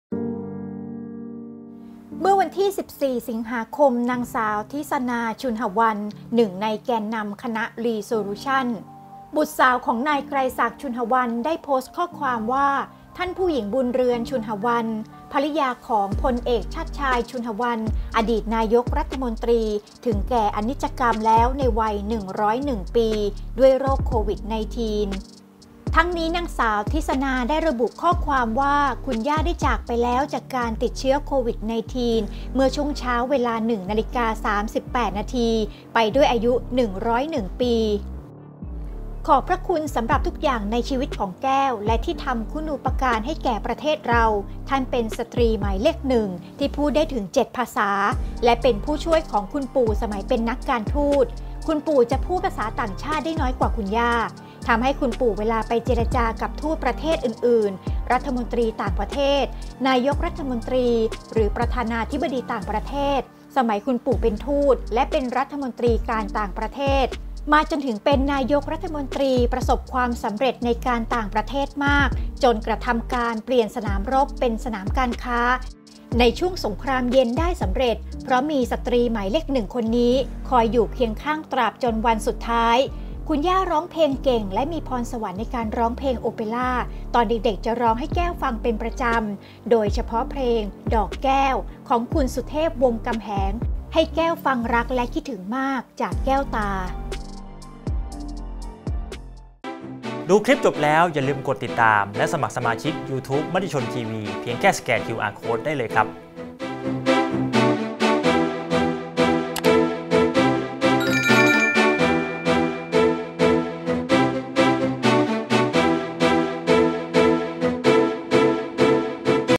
"ท่านผู้หญิงบุญเรือน ชุณหะวัณ" ภริยาอดีตนายกฯ "ชาติชาย" ติดโควิดถึงแก่อนิจกรรมในวัย101ปี:Matichon TV
น.ส.ธิษะณา ชุณหะวัณ หนึ่งในแกนนำคณะ Re-solution บุตรสาวของ นายไกรศักดิ์ ชุณหะวัณ ได้โพสต์ข้อความเผยว่า ท่านผู้หญิงบุญเรือน ชุณหะวัณ ภริยาของ พล.อ.ชาติชาย ชุณหะวัณ อดีตนายกรัฐมนตรี ถึงแก่อนิจกรรมแล้วในวัย 101 ปี ด้วยโรคโควิด